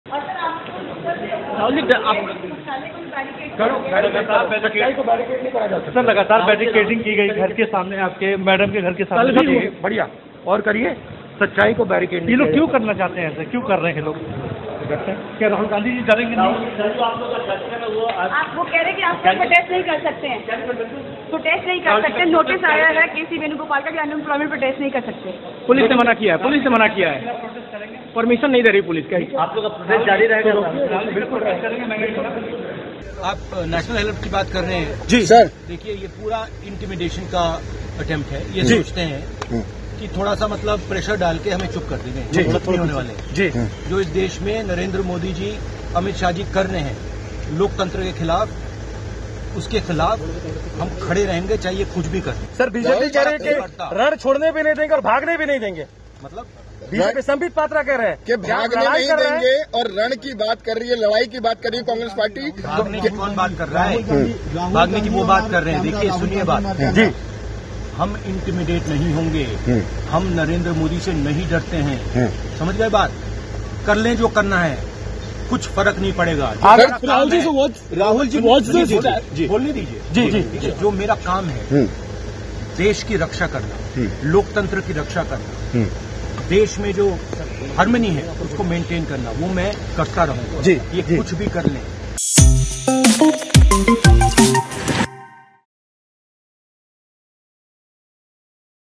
Rahul Gandhi ने Narendra Modi को लेकर कह दी बड़ी बात, बोले- कर ले जो.....
कांग्रेस (Congress) के पूर्व अध्यक्ष और वायनाड सांसद राहुल गांधी (Rahul Gandhi) ने नेशनल हेराल्ड (National Herald) बिल्डिंग स्थित यंग इंडिया लिमिटेड के दफ्तर को सील किए जाने और दिल्ली स्थित कांग्रेस दफ्तर (Congress Headquarters) के बाहर सुरक्षा बढ़ाए जाने पर बयान दिया है. राहुल गांधी ने कहा कि उन्हें डराने की कोशिश की जा रही है लेकिन वह डरने वाले नहीं है. राहुल गांधी ने कहा, ''सच्चाई को बैरिकेड नहीं किया जा सकता.'' राहुल ने प्रधानमंत्री नरेंद्र मोदी (Narendra Modi) और गृहमंत्री अमित शाह (Amit Shah) पर गंभीर आरोप लगाते हुए एक बार फिर कहा कि मोदी-शाह लोकतंत्र (Democracy) के खिलाफ काम कर रहे हैं. राहुल ने कहा कि वह लोकतंत्र की रक्षा के लिए लड़ते रहेंगे.